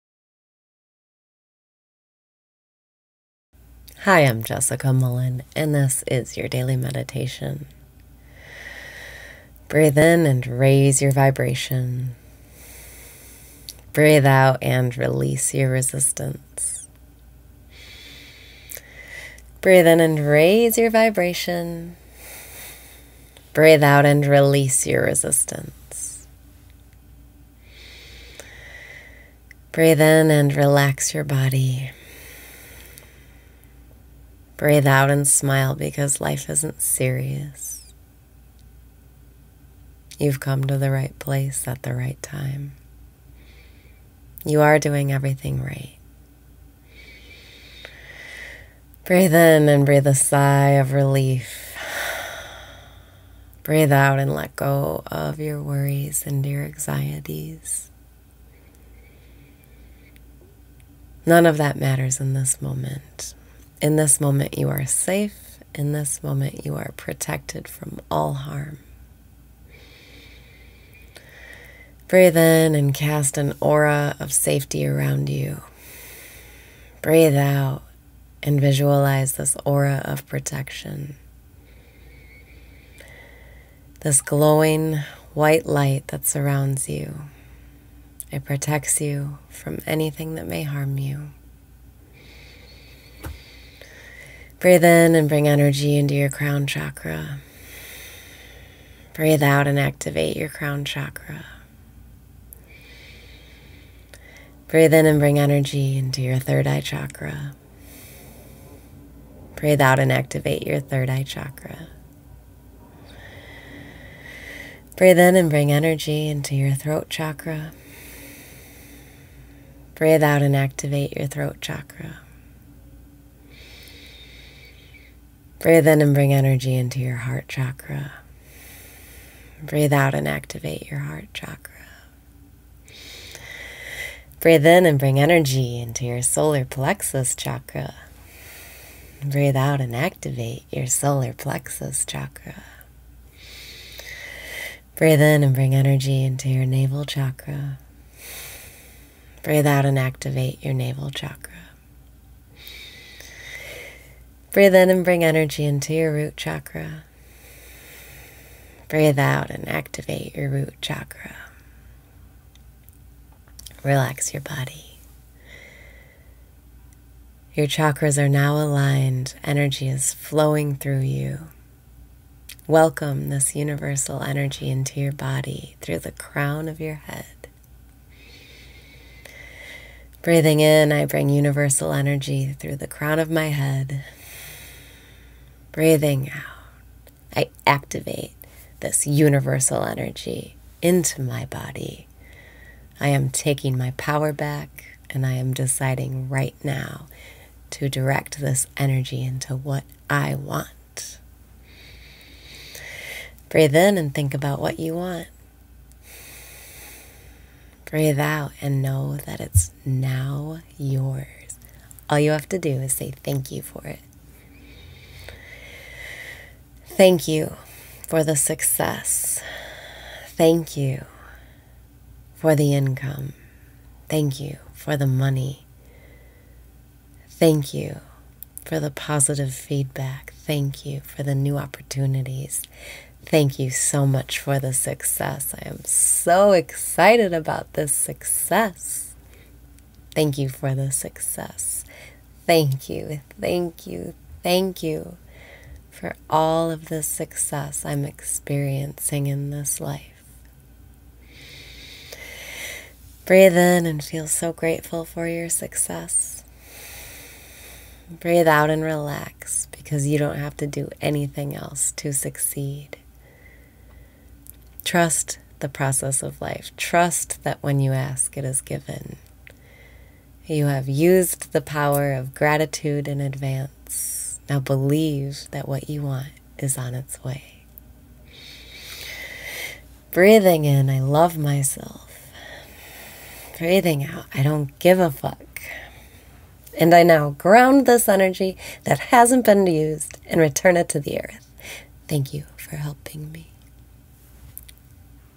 Daily Meditation - Aura of Protection
Breathe out and smile because life isn’t serious!

You’ve come to the right place at the right time. You are doing everything right.

Breathe in and breathe a sigh of relief.
Breathe out and let go of your worries and your anxieties.

None of that matters in this moment. In this moment you are safe. In this moment you are protected from all harm.

Breathe in and cast an aura of safety around you.
Breathe out and visualize this aura of protection.

This glowing white light that surrounds you—it protects you from anything that may harm you.

Breathe in and bring energy into your crown chakra.
Breathe out and activate your crown chakra.

Breathe in and bring energy into your third eye chakra.
Breathe out and activate your third eye chakra.

Breathe in and bring energy into your throat chakra.
Breathe out and activate your throat chakra.

Breathe in and bring energy into your heart chakra.
Breathe out and activate your heart chakra.

Breathe in and bring energy into your solar plexus chakra.
Breathe out and activate your solar plexus chakra.

Breathe in and bring energy into your navel chakra.
Breathe out and activate your navel chakra.

Breathe in and bring energy into your root chakra.
Breathe out and activate your root chakra.

Relax your body. Your chakras are now aligned. Energy is flowing through you. Welcome this universal energy into your body through the crown of your head.

Breathing in I bring universal energy through the crown of my head.
Breathing out I activate this universal energy into my body.

I am taking my power back and I am deciding right now to direct this energy into what I want!

Breathe out and know that it’s now yours.

All you have to do is say thank you for it.

Thank you for the success. Thank you for the income. Thank you for the money. Thank you for the positive feedback. Thank you for the new opportunities. Thank you so much for the success. I am so excited about this success! Thank you for this success! Thank you, thank you, thank you, for all of this success I’m experiencing in this life!

Breathe in and feel so grateful for your success.
Breathe out and relax because you don’t have to do anything else to succeed.

Trust the process of life. Trust that when you ask, it is given. You have used the power of gratitude in advance. Now believe that what you want is on its way.

and I now ground this energy that hasn’t been used and return it to Earth. Thank you for helping me!